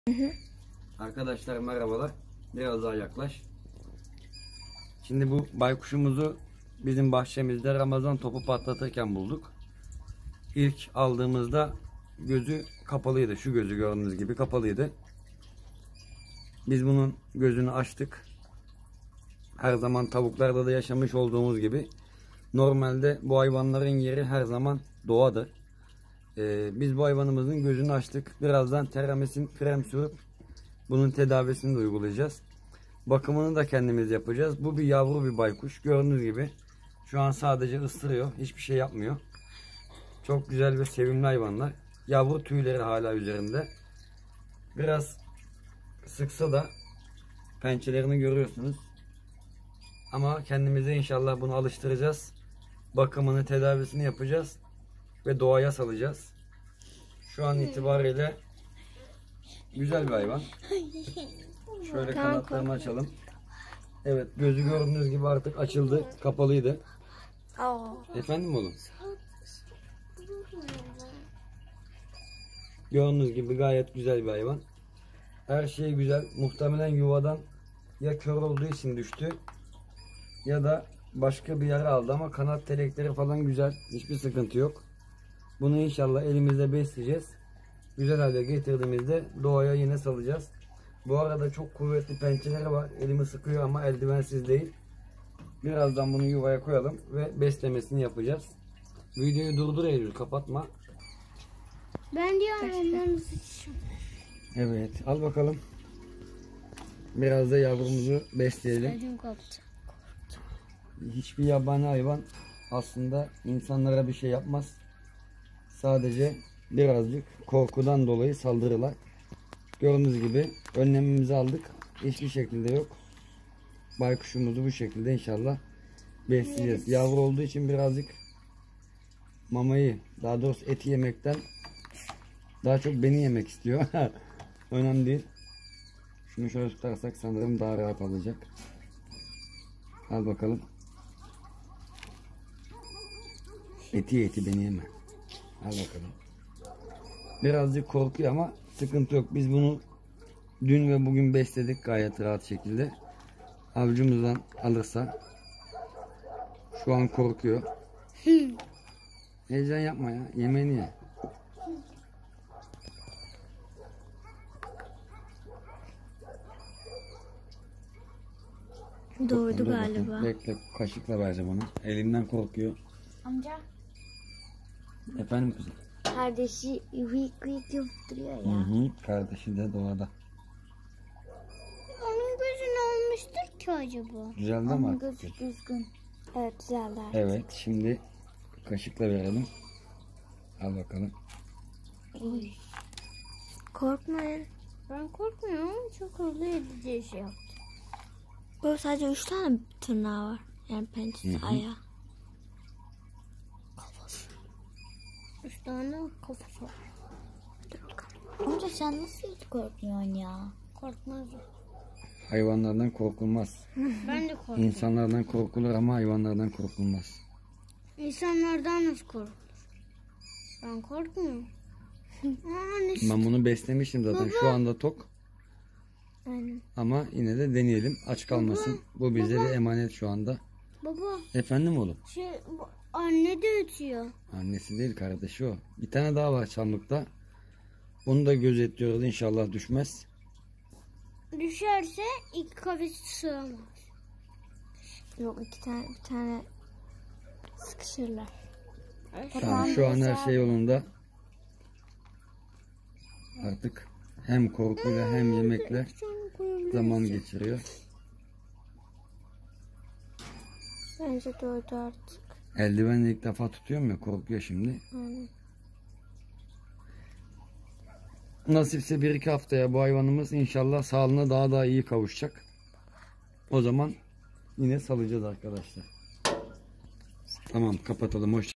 BAYKUŞ YAVRUSUNU TEDAVI EDIP DOGAYA SALACAGIZ
Bahçede  bulduğumuz kör olan baykuş yavrusunu tedavi ediyoruz.